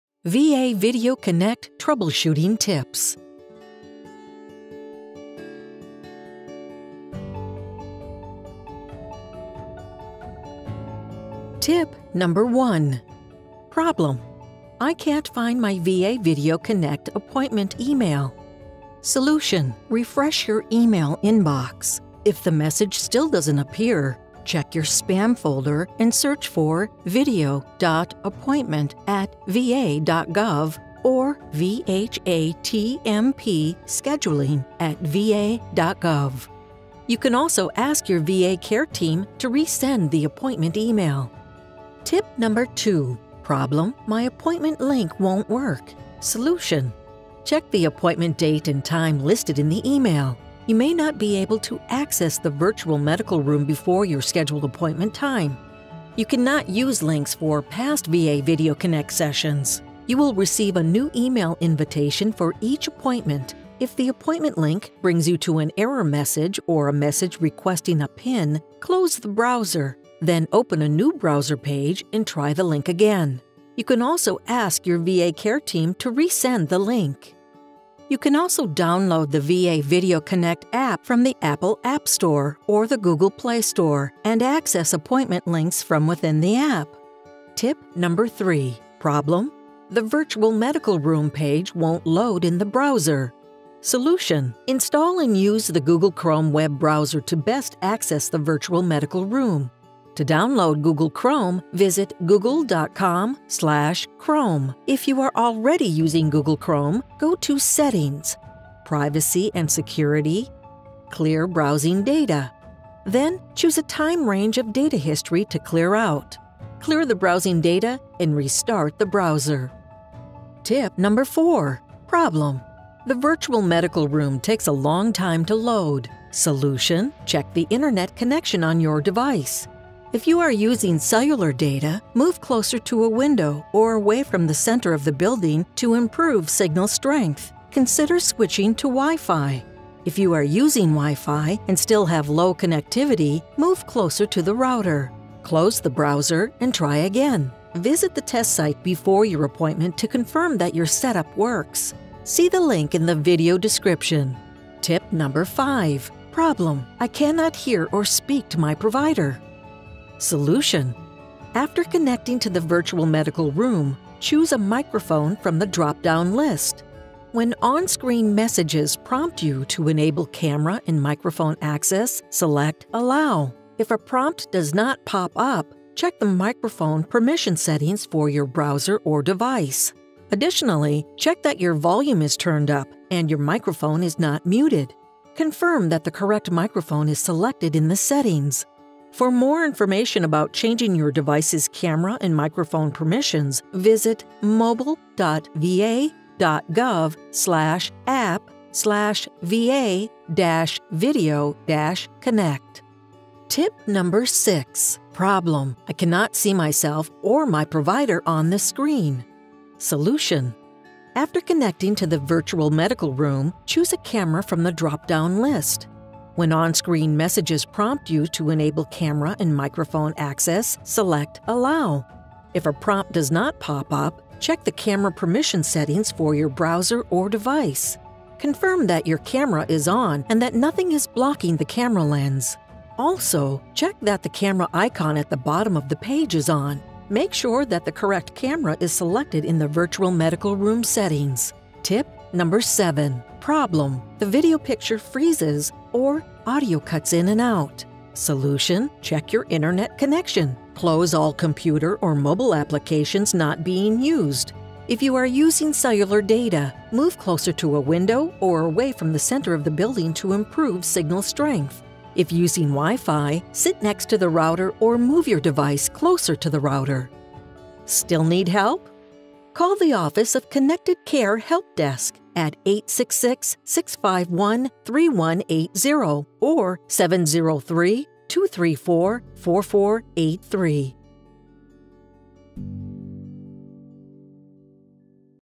VA Video Connect Troubleshooting Tips
VA Video Connect allows you to quickly and easily meet with your care team through live video on any computer, tablet, or mobile device with an internet connection. In this video, you'll learn tips for troubleshooting issues you may encounter when using VA Video Connect.

TIP 1 - I can’t find my VA Video Connect appointment email. (0:11)
TIP 2 - My appointment link won’t work. (0:42)
TIP 3 - The virtual medical room page won’t load in the browser. (1:18)
TIP 4 - The virtual medical room takes a long time to load. (1:51)
TIP 5 - I cannot hear or speak to my provider. (2:20)
TIP 6 - I cannot see myself or my provider on the screen. (3:09)
TIP 7 - The video picture freezes or audio cuts in and out. (3:47)